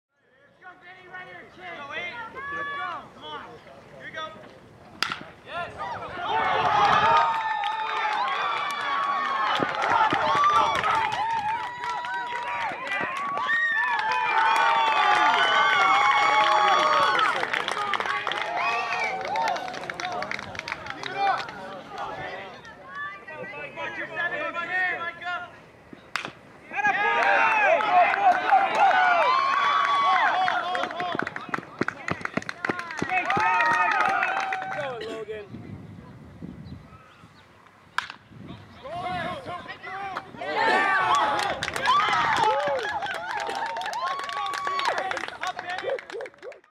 Thousand Oaks Knights - Four Run Rally - Southwest Wood Bat Fall Classic - 10-4-2020
Thousand Oaks Knights rally to score four runs in the top of the sixth inning, as Vinny Neilson hits a line drive single to left field to score Gavin Arce and Jack Medhurst, Micah Rodriguez singles to right field to score Eli Steinhaus, and Logen Seeger singles on a ground ball to the left side to score Neilson.